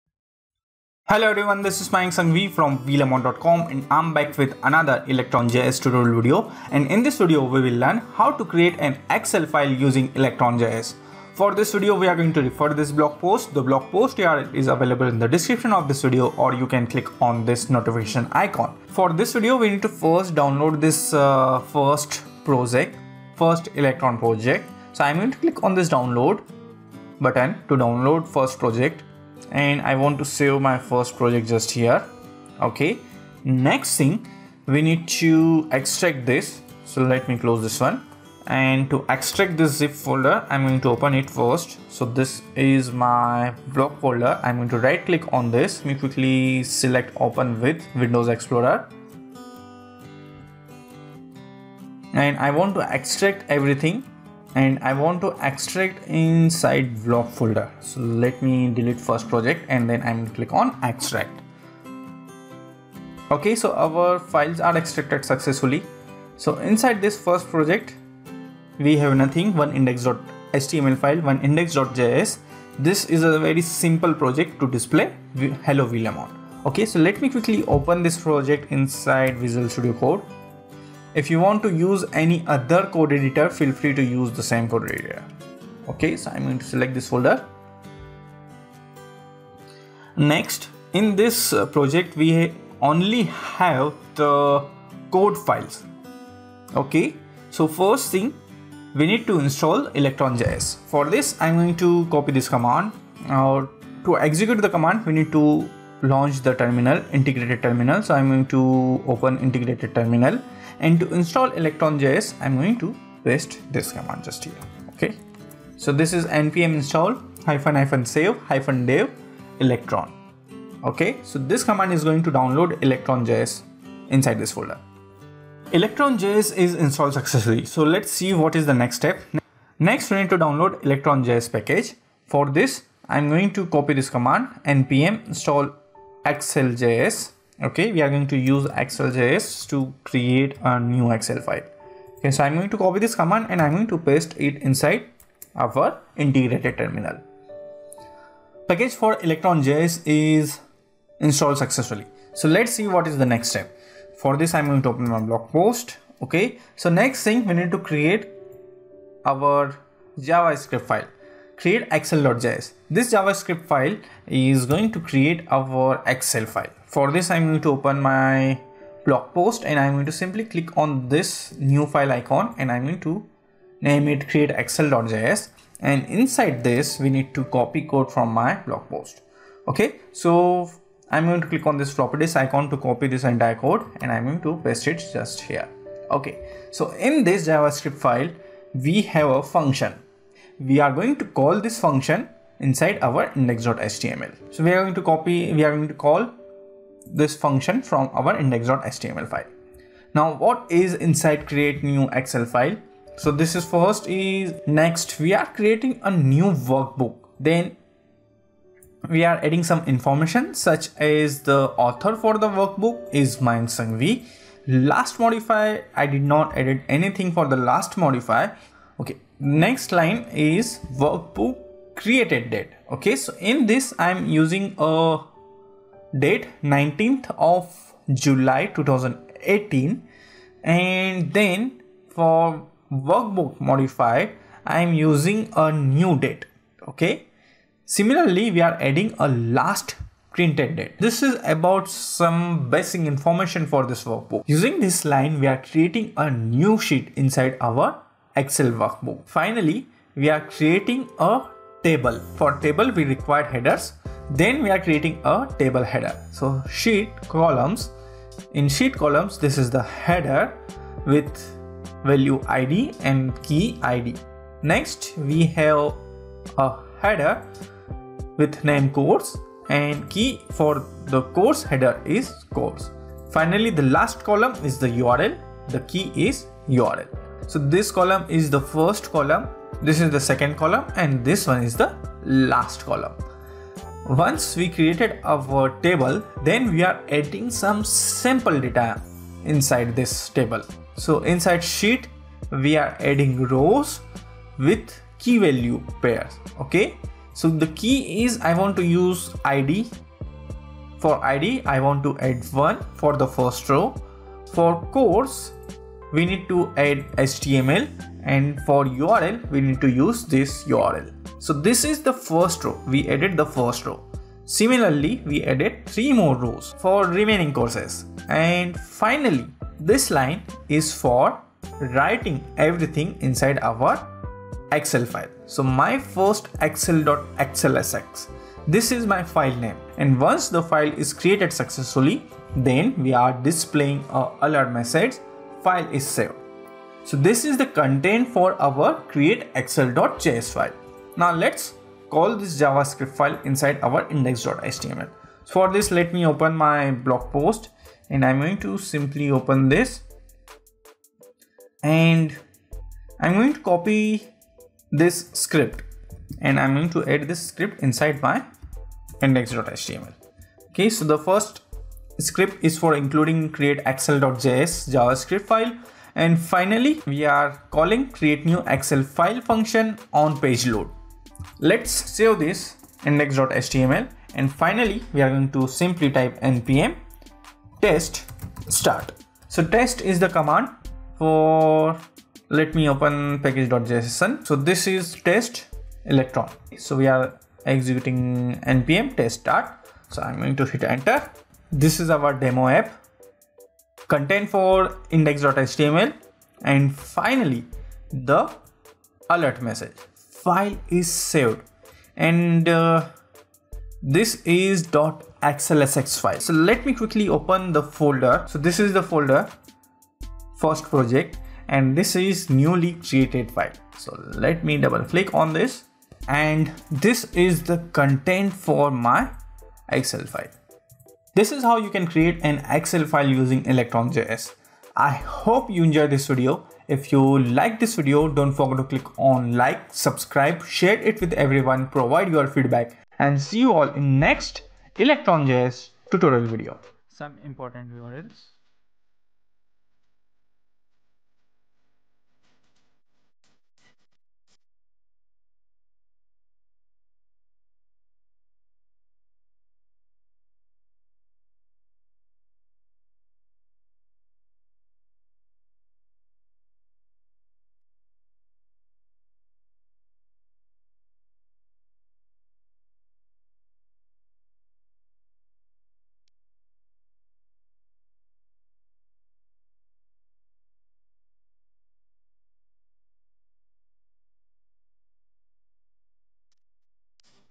How to Create an Excel File Using Electron JS (Node.js)
In this video, we will learn How to create an Excel file with Electron JS? In order to create an Excel file we need to download a package for Excel. We have multiple options in npm, but we use ExcelJS for this article. For our demo app we will first download our Electron JS Tutorial First Project.

Hire Me: Charges may apply.